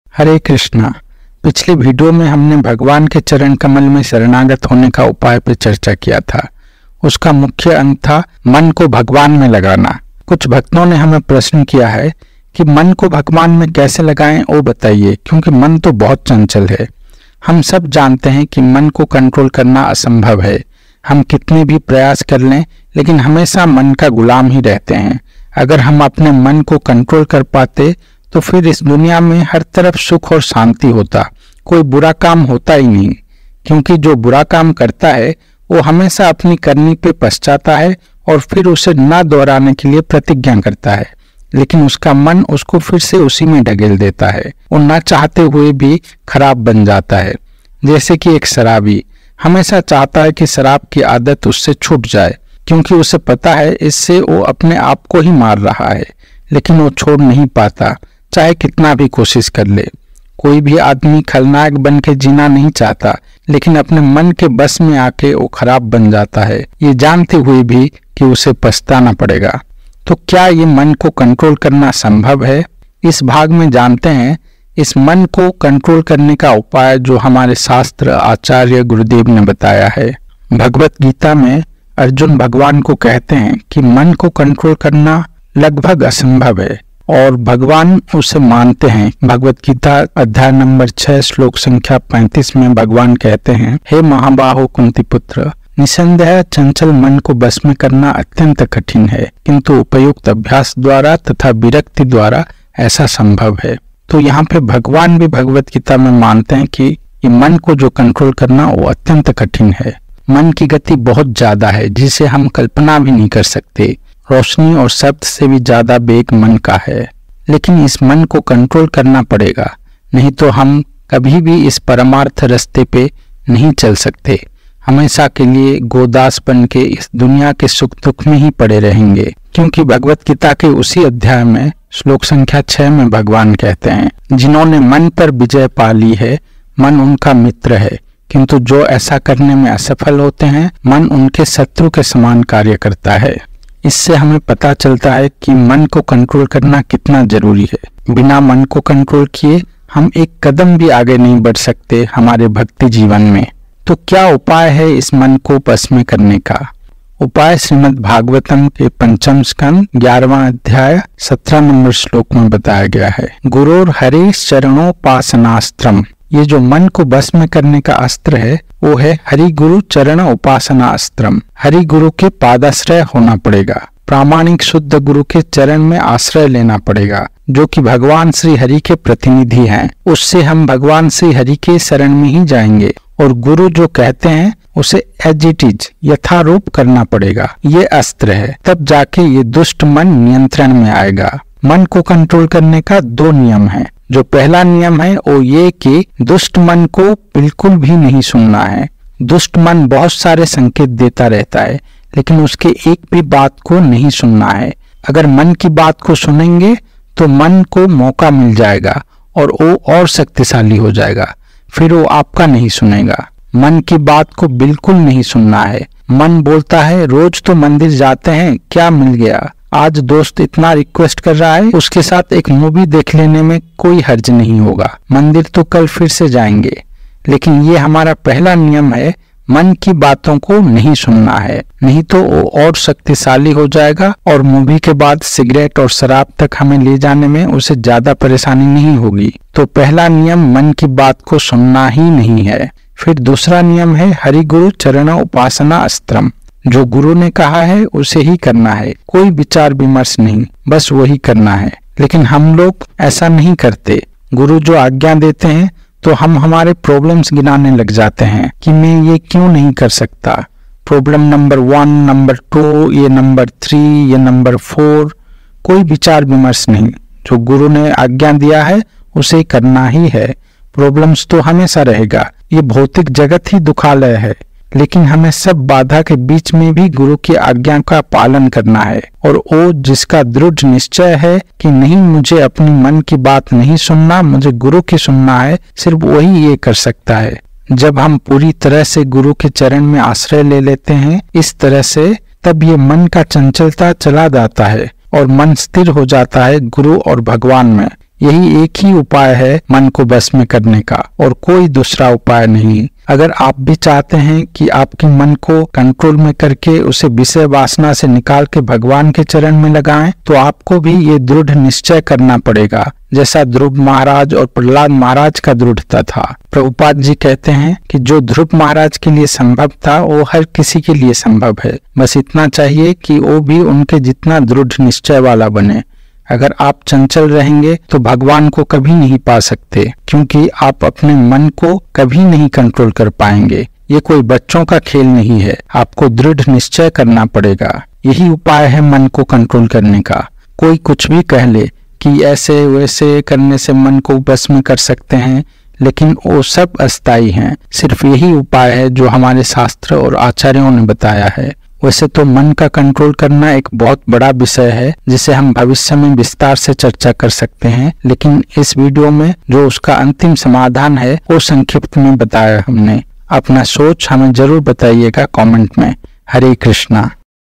मन को कैसे जीतें (How to Control the Mind)
Hare Krishna
Bhagavat Gita says Mind is our best of friend and the worst of enemy. One who can control the mind he can rule over this world, truly. So in this short video we get to know how we can control our mind and thus live a peaceful life here in this world and afterwards go back to the Godhead,
This video summarizes all the possible ways to control our mind in two rules which are given in the Srimad Bhagavatam and explained by Our Acharyas.
Please give your suggestions and questions in the comments.

Thanking You
In the service of Guru and Gouranga
Sri Guru Hridaya Deva Das